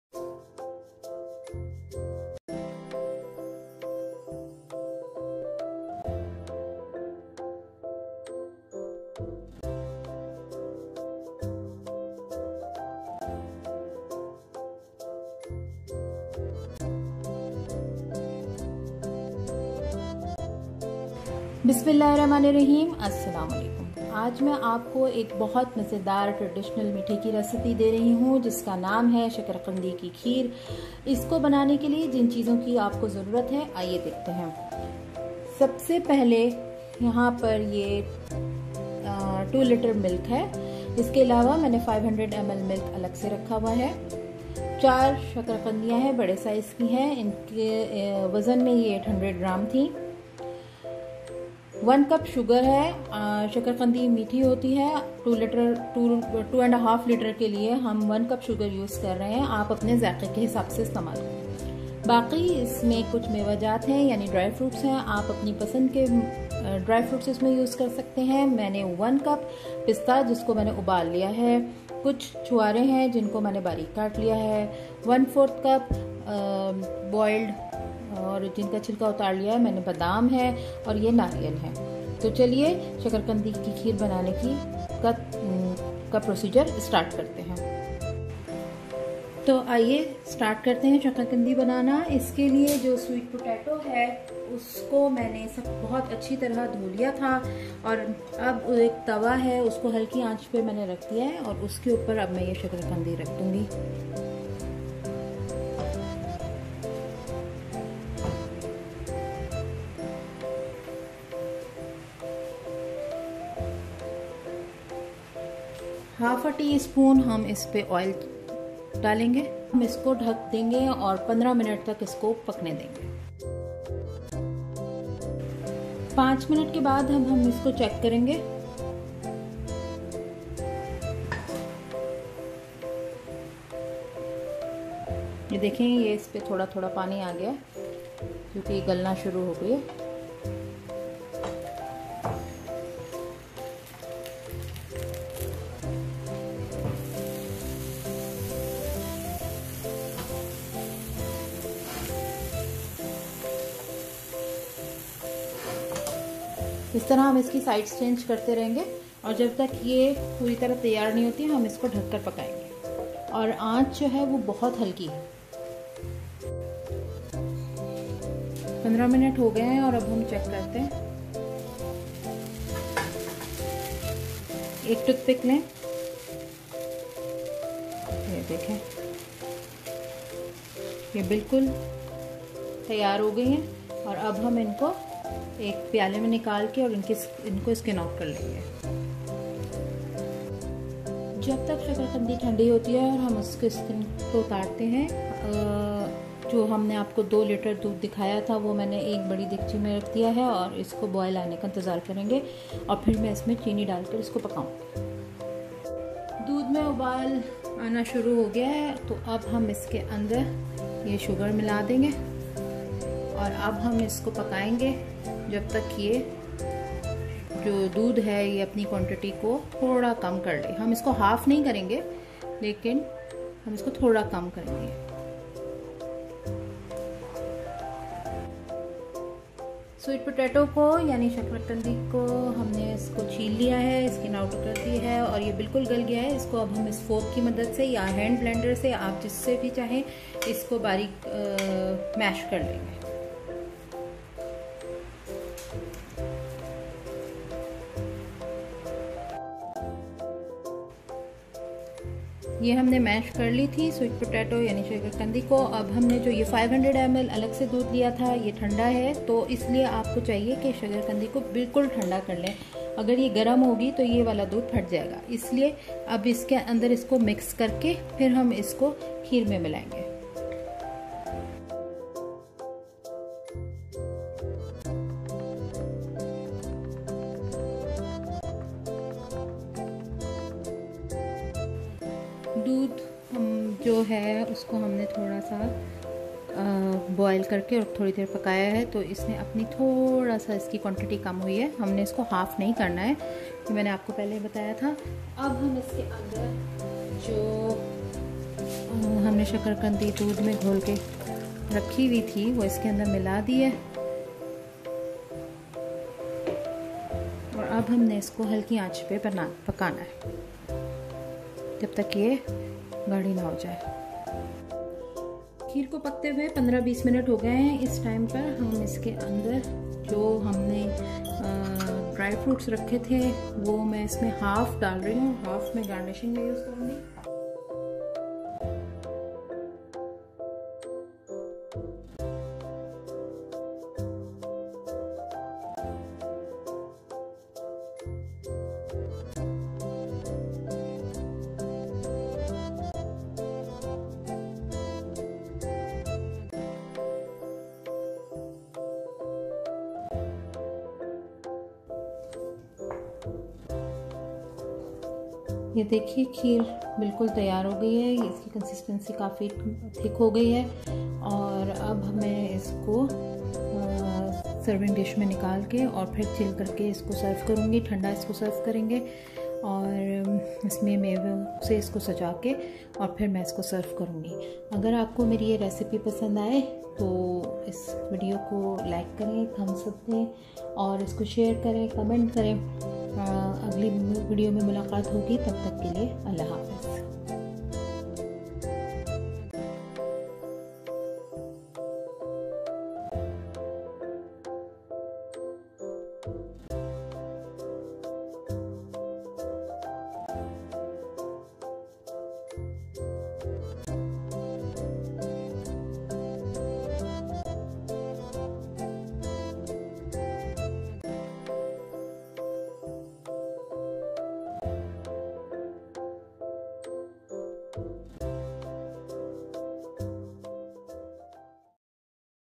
SWEET POTATO PUDDING/KHEER/MAZEDAR EID MEETHA شکر قندی کی کھیر/
When we think of Eid e Qurban, the first thing comes in mind is GOSHT, but when it comes to Eid, it is always MEETHA. I remember my mom just after cooking kaleji used to make any dessert for us. It is a tradition which is so beautiful.

I made shakar qandi ki kheer. You can take my words that if you make it once, you will be hooked to it.
Tomorrow is Eid and its a good chance to give it a try. Eid Mabrook